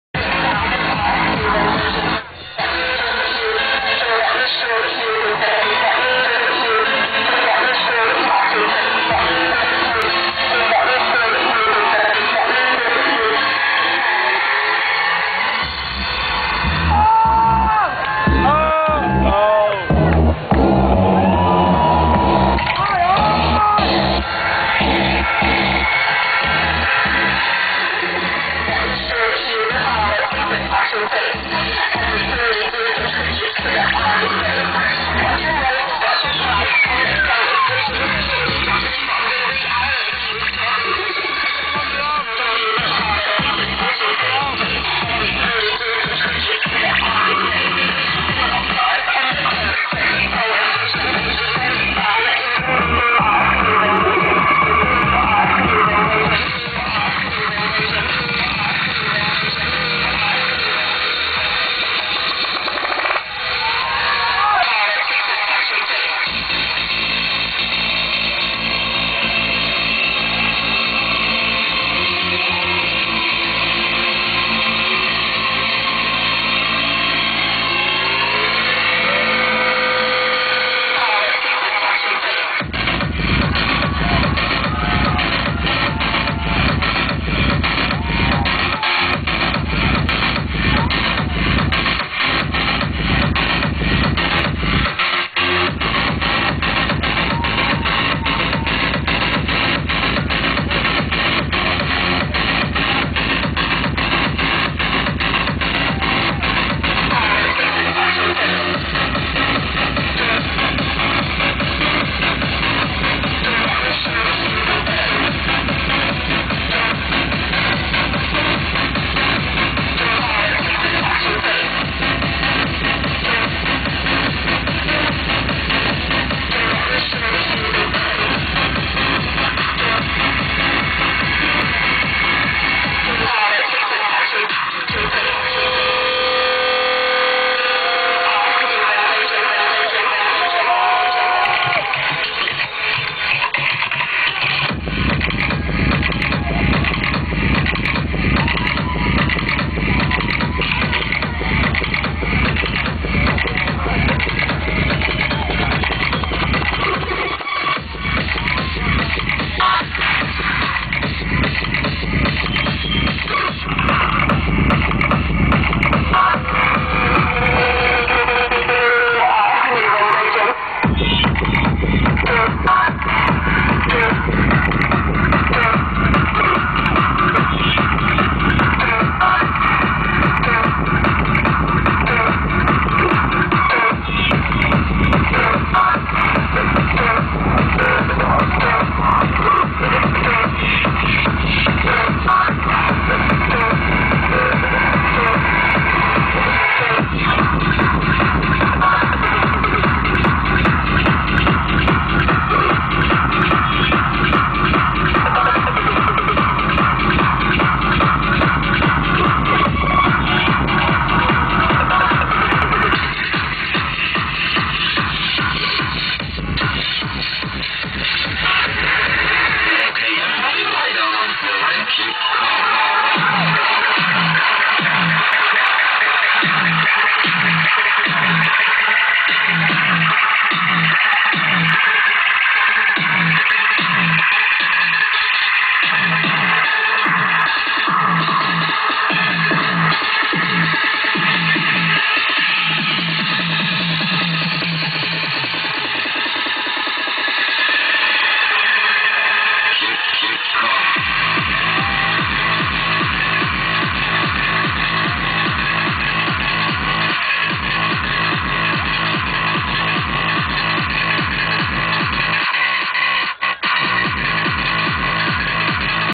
global gathering 2010 Lisa Lashes
Lisa Lashes, Polysexual stage Global Gathering 2010. So sorry about low qualiy but I fluffed it